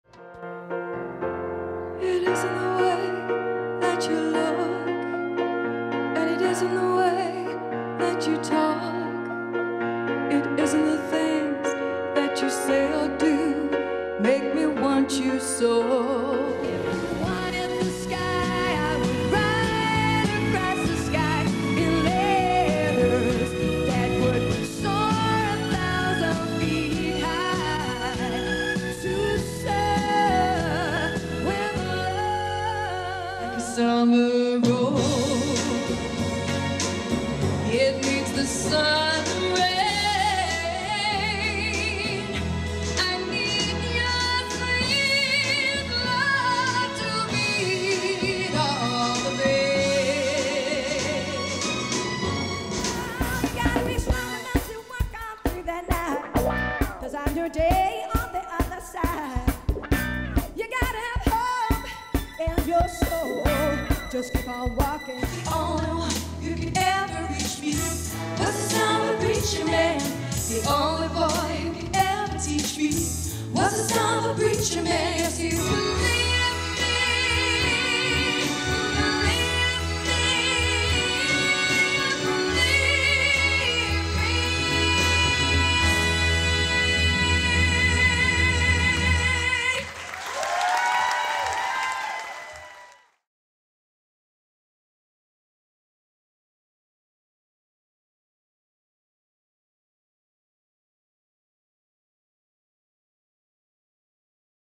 Maxine Mazumder as Dusty Springfield and Lulu
Maxine Mazumder is a UK tribute act who performs as 2 iconic singers from the 60's. Her stunningly accurate vocal portrayal is equally matched by an entertaining and exciting show.